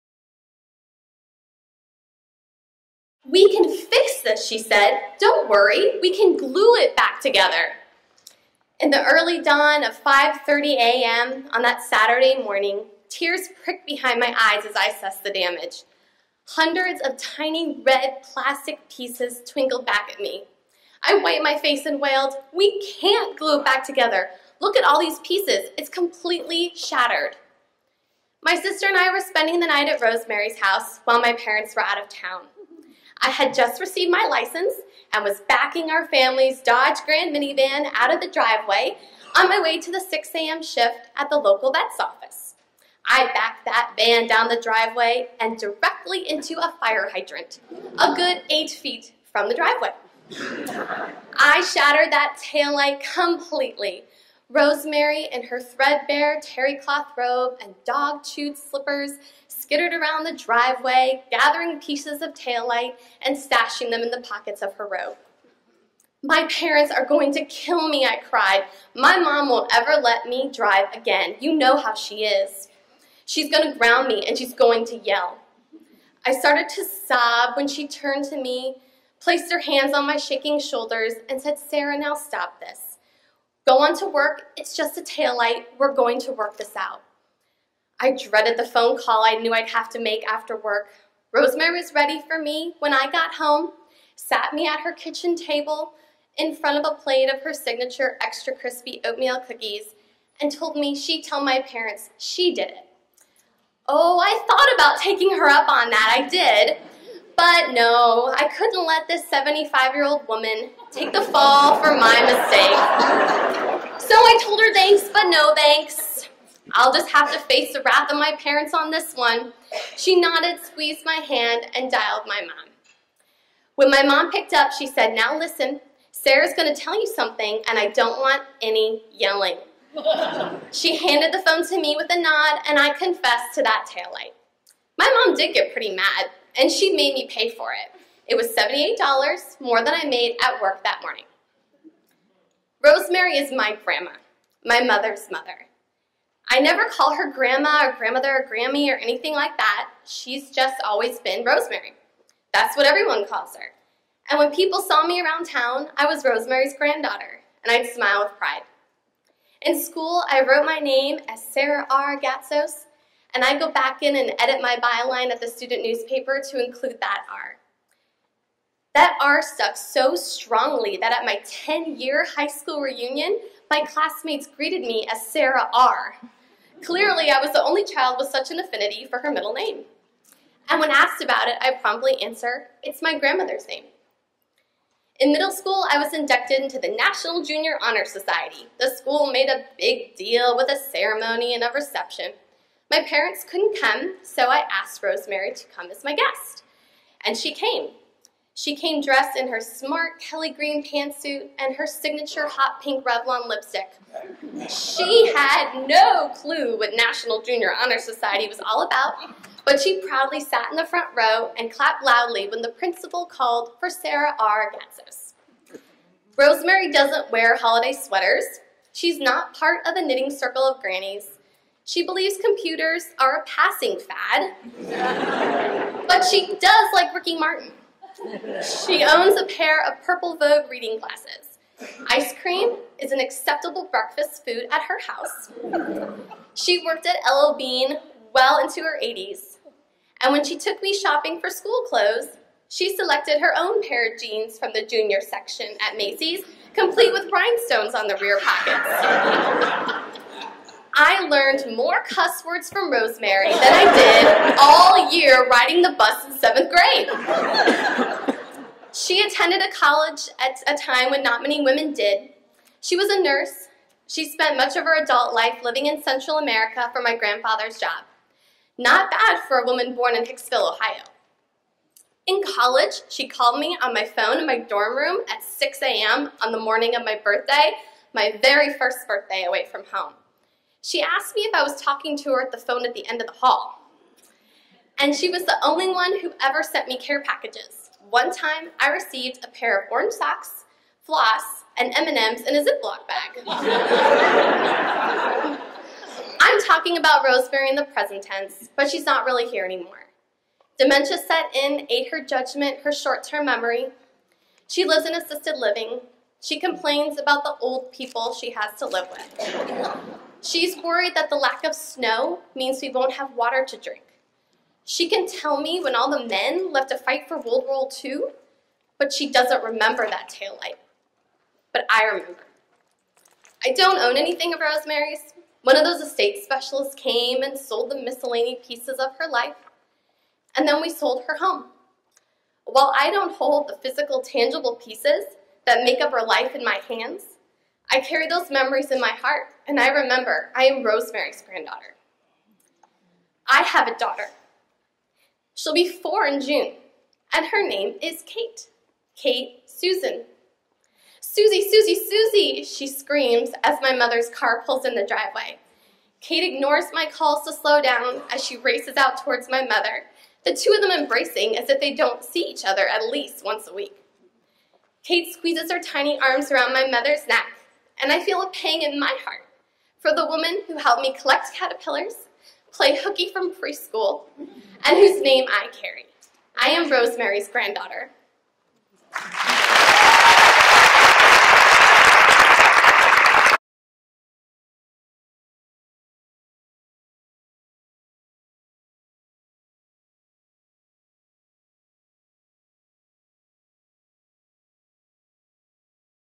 Sarah R. Bagley reading "Rosemary's Granddaughter"
Sarah proudly honors the memory of her non-traditional grandmother.

LTYM: Baltimore ● 4/26/14
Harry & Jeanette Weinberg Auditorium, Mercy High School